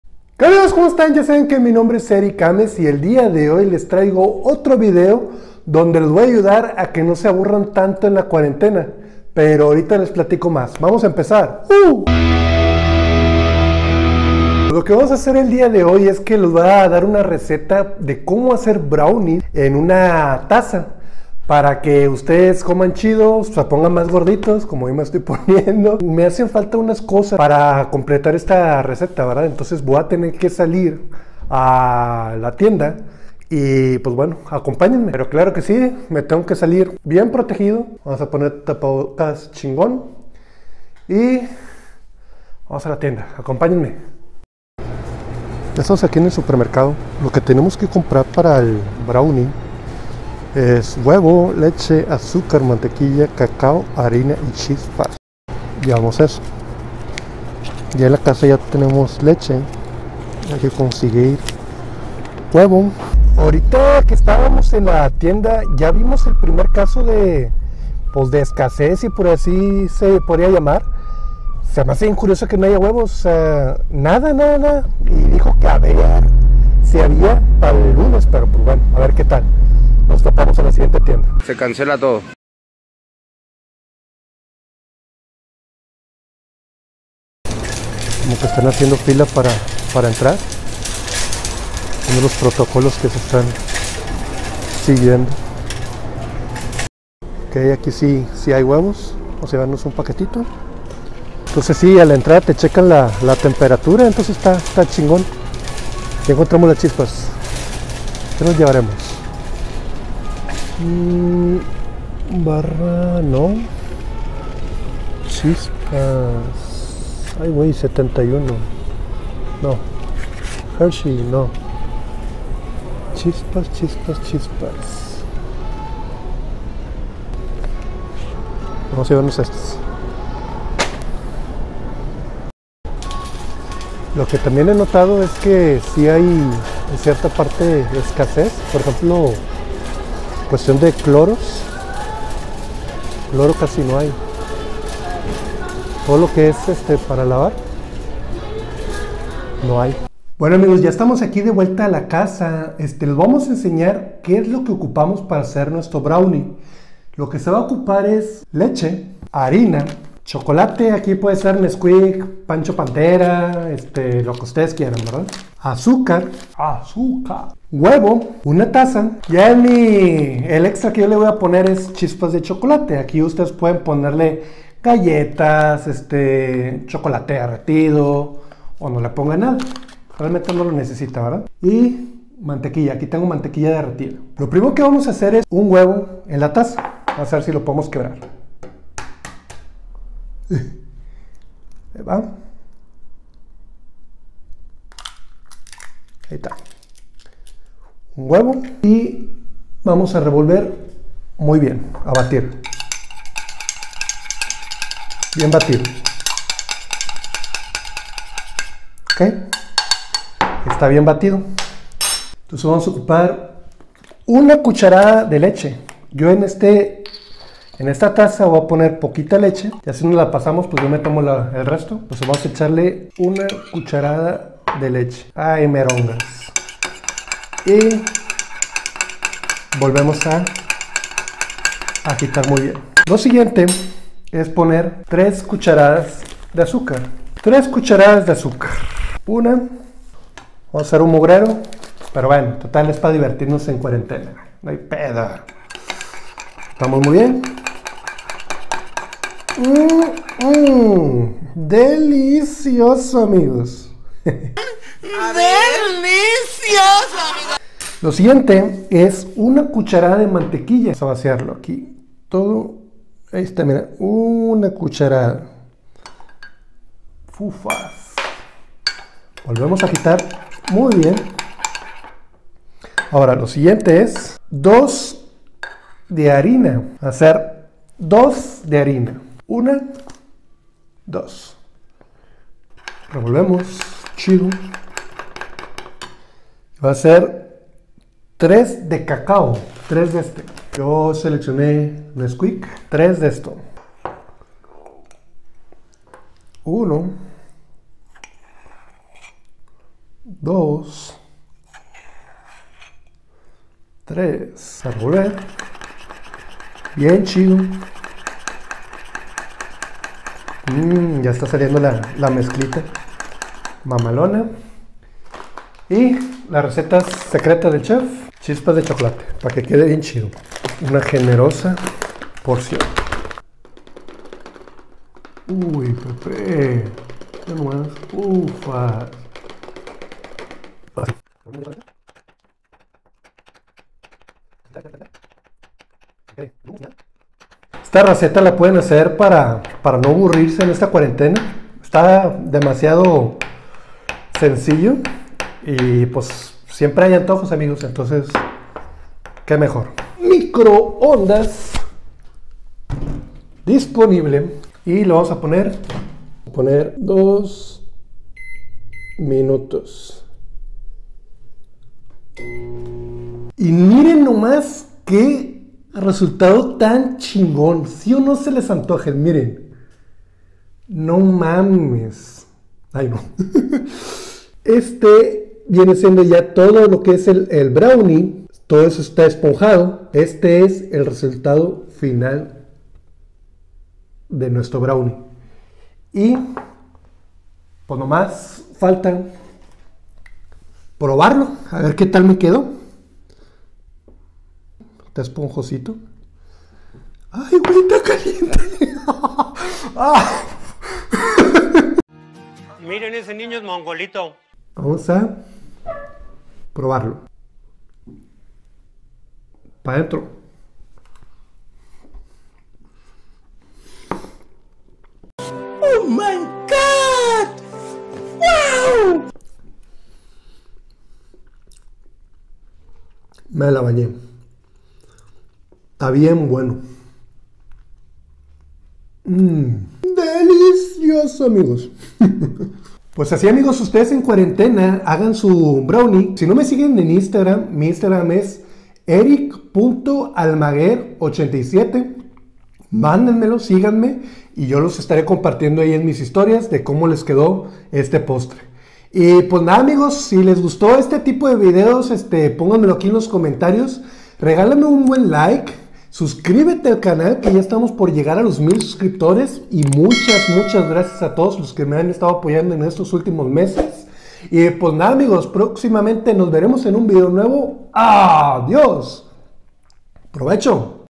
COMO HACER BROWNIES EN MICROONDAS EN 2 MINUTOS | COCHINANDO CON ERICK
Amigos les dejo un video diferente para que se diviertan en cuarentena, es el primer COCHINANDO CON ERICK, la receta de como hacer brownies en microondas en tan solo 2 minutos, y sabeeeen chingon, suscribanse al canal amigos ya casi somos 1000 suscriptores.